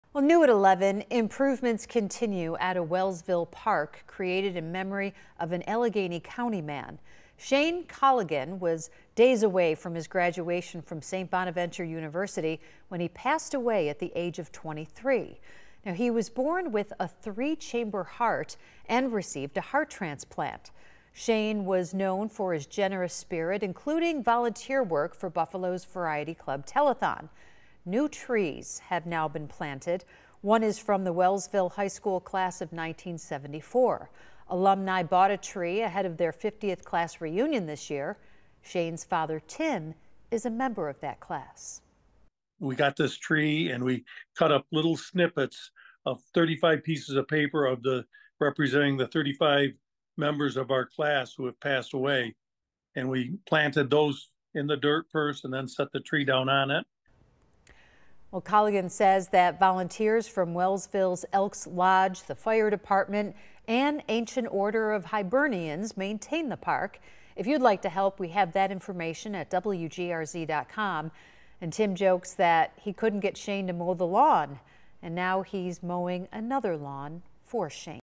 Memorial park for Wellsville man spruced up with new trees
Shane Colligan passed away just days before he was to graduate from St. Bonaventure.